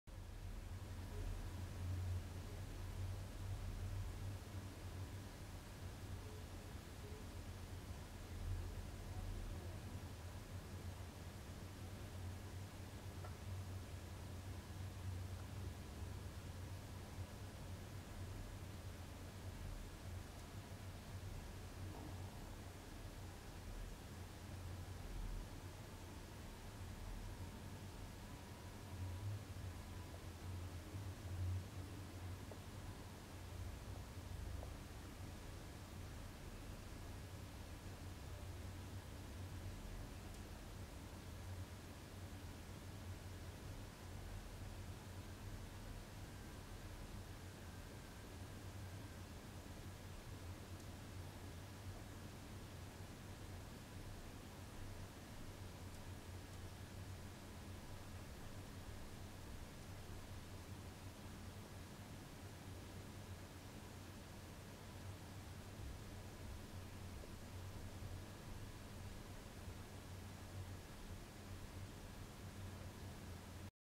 Peacock Butterfly drinking honey water (take #2)
European Peacock Butterfly (Inachis io) drinking honey water.

Neitoperhonen (Inachis io) juo hunajavettä.